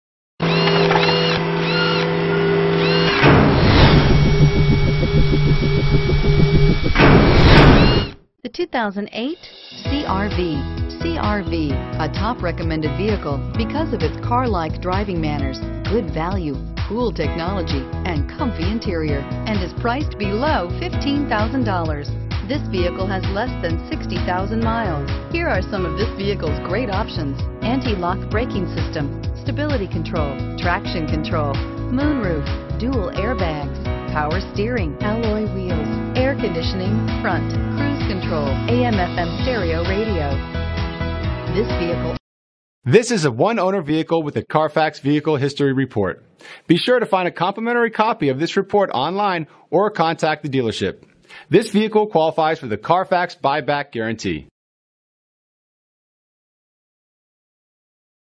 2008 Honda CR-V used, Mazda 3, Prius, Sienna, Grand Cheoroke, Impreza, Burlingame, Bay Area, Penins
2008 Honda CR-V EX

Putnam Automotive
3 California Drive

You'll love this 2008 Honda CR-V. This is a SUV you'll want to take home. With 59745 miles, it features Automatic transmission and an exterior color of Blue. Call and get in touch with Putnam Automotive directly, and be the first to open the SUV door today!